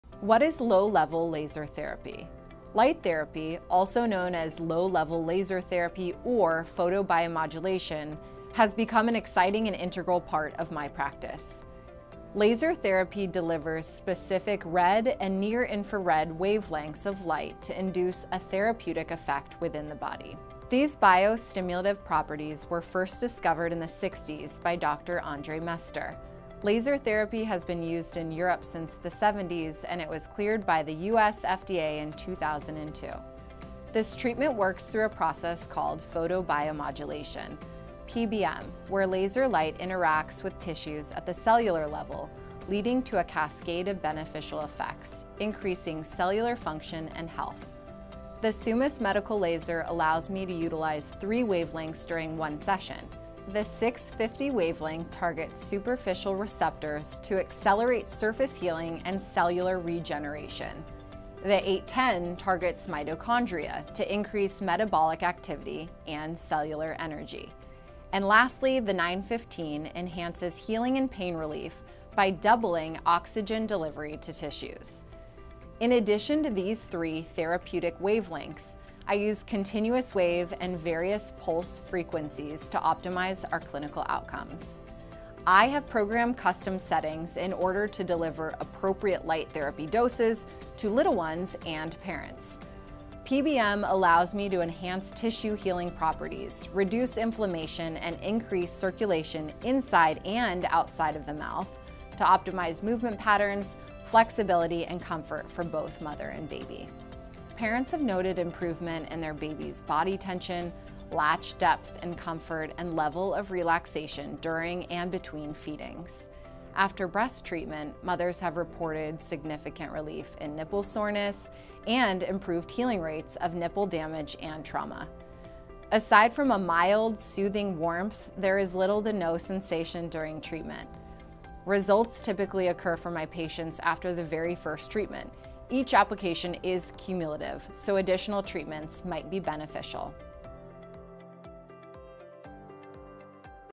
What is Low Level Laser Therapy?
Light therapy (also known as low level laser therapy or photobiomodulation) has become an exciting and integral part of Dr. Pinto's practice.

Laser therapy delivers specific red and near infrared wavelengths of light to induce a therapeutic effect within the body. These biostimulative properties were first discovered in the 60’s by Dr. Endre Mester. Laser therapy has been used in Europe since the 70s… and it was cleared by the US FDA in 2002.

This treatment works through a process called photobiomodulation (PBM), where laser light interacts with tissues at the cellular level, leading to a cascade of beneficial effects – increasing cellular function and health.

PBM allows Dr. Pinto to enhance tissue healing properties, reduce inflammation and increase circulation inside and outside of the mouth to optimize movement patterns, flexibility and comfort for both mother and baby. Aside from a mild, soothing warmth, there is little to no sensation during treatment.